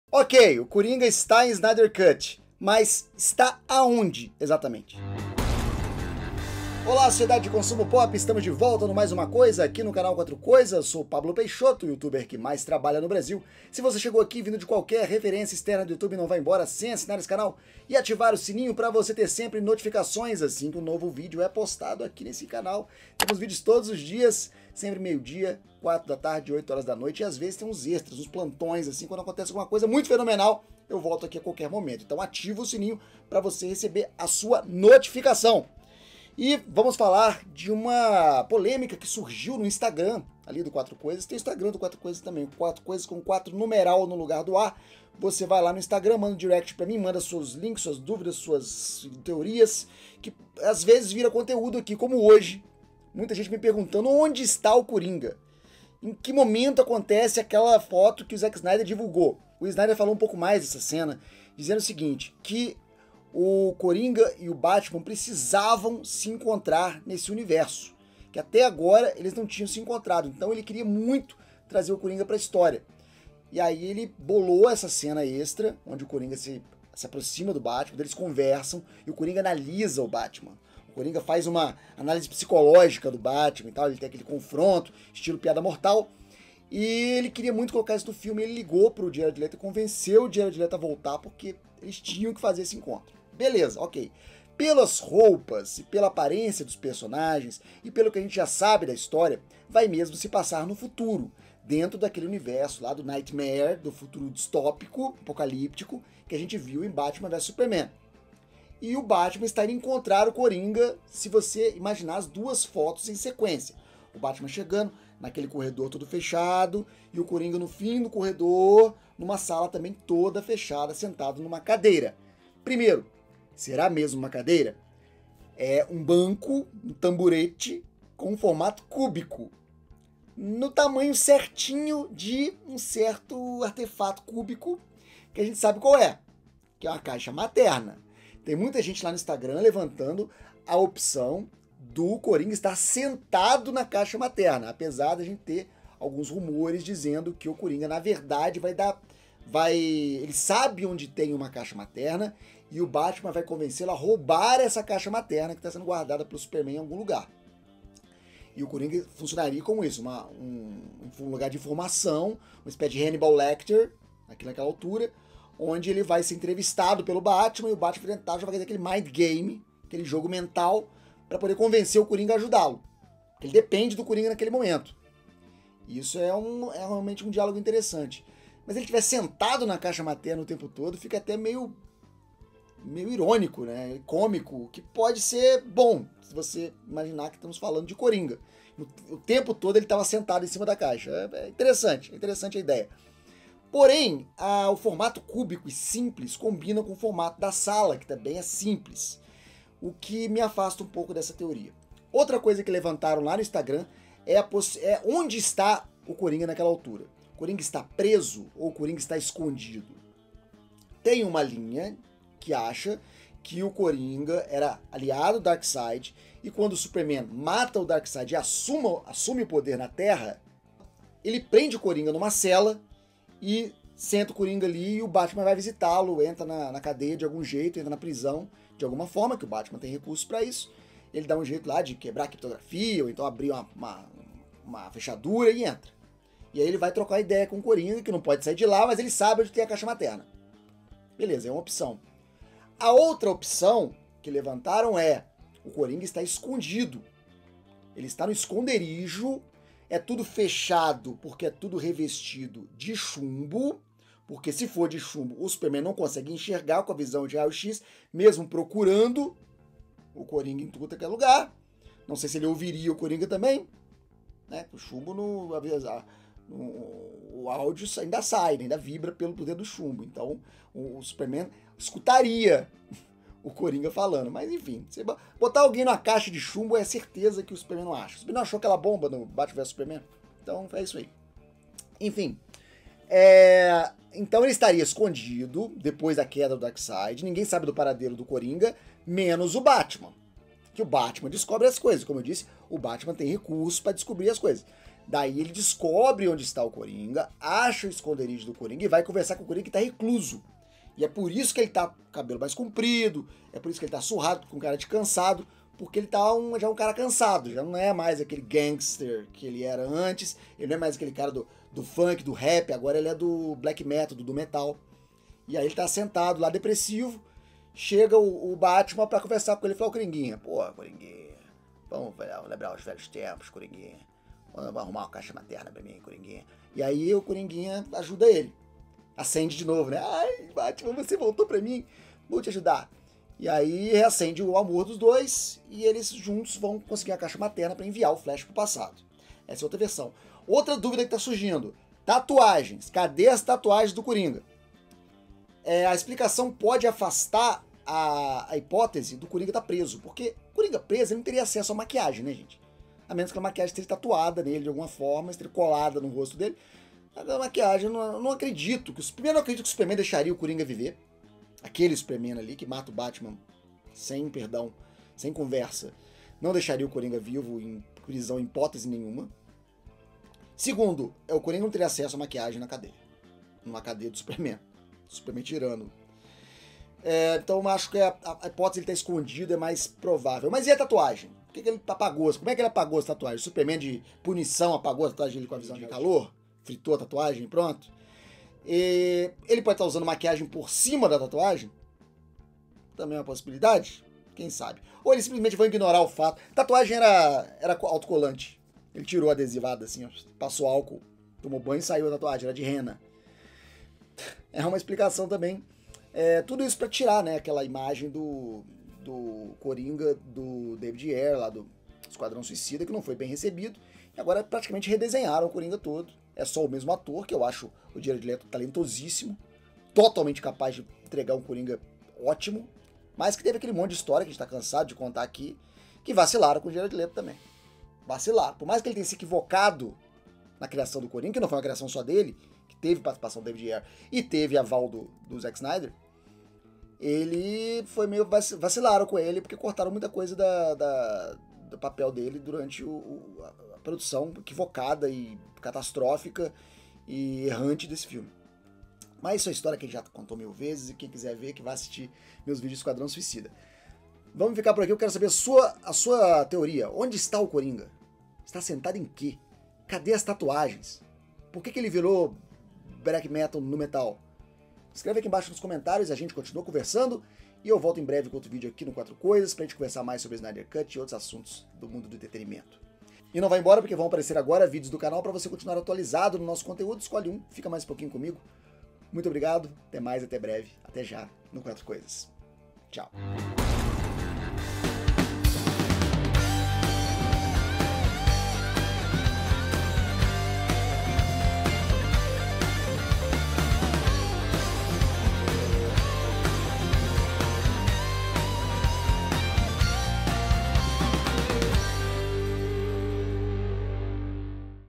EU SEI ONDE ESTÁ O CORINGA NESSE MOMENTO E VOU TE CONTAR!
Pablo Peixoto do Qu4tro Coisas fala sobre teorias sobre quando e onde o Coringa está esperando o batman do Snydercut